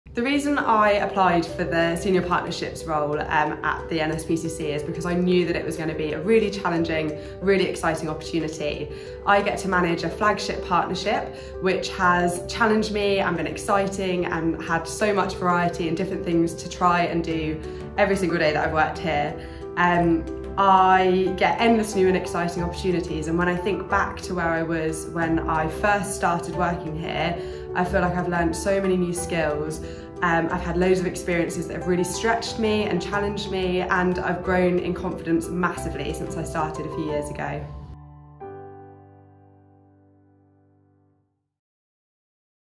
A Place for Positive Change - Employee Stories: Bethany Major
Our people come from all walks of life, and work across a huge and diverse range of disciplines. But they all have these things in common - they put children first in all they do, are deeply connected to our cause and share our values. Find out more about their roles and why they love them.